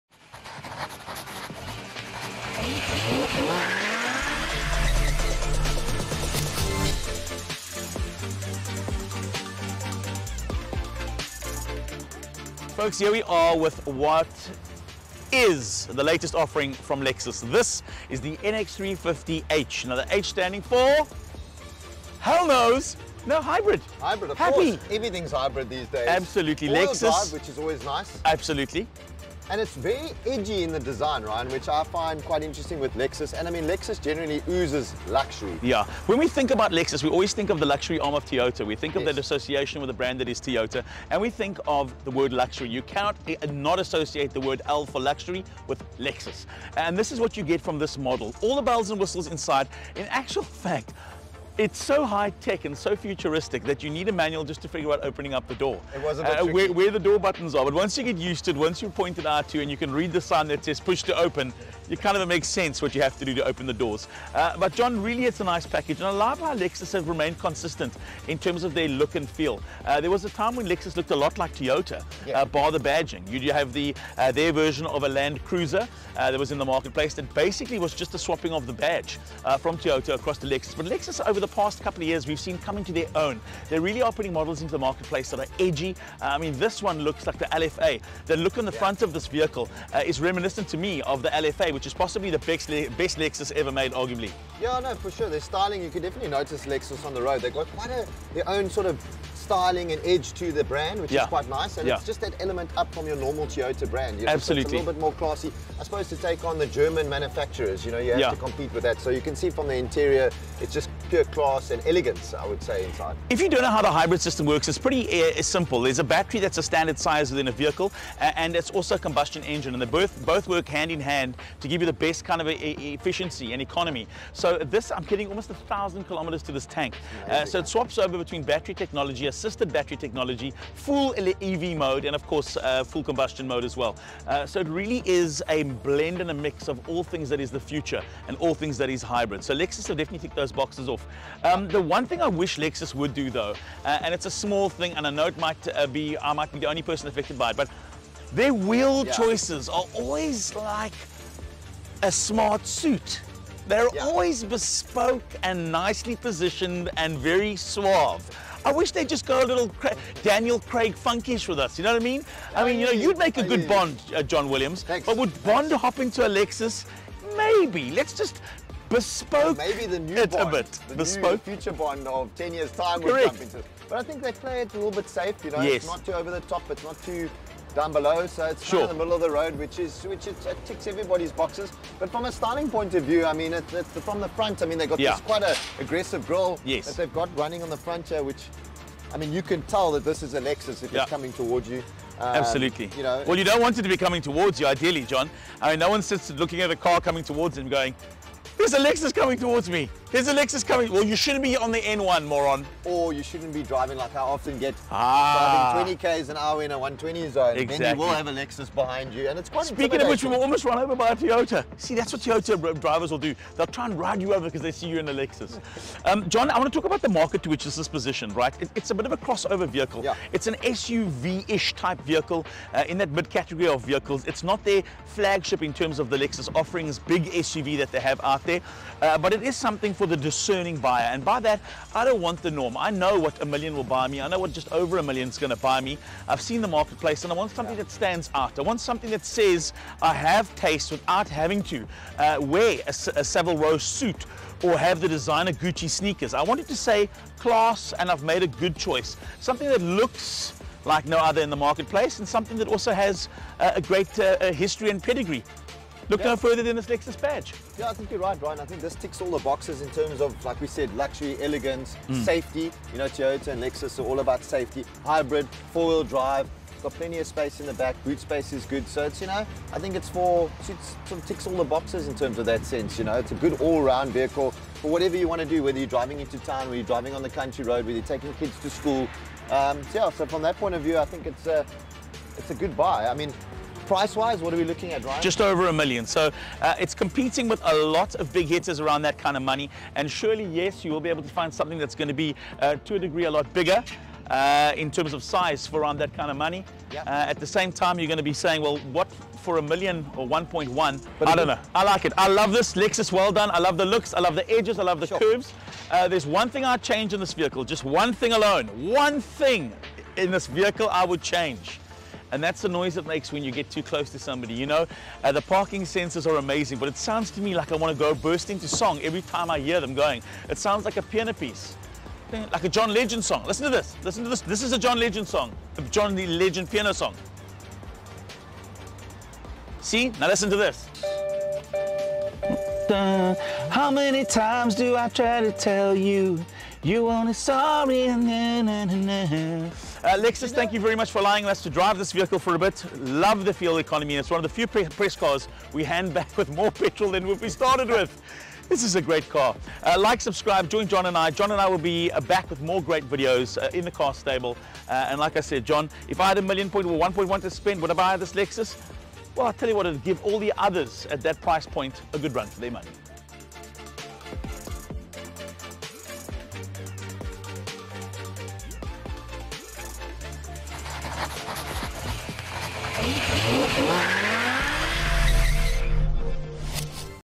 ROC Talks Through His Time with the Ultra-Frugal Lexus NX 350h
After spending some time with the all-new Lexus NX350h, ROC talks through some highlights of the luxury SUV which can manage over 1 000 km per tank and starts from just over R1 million.

Want to get your hands on some new wheels?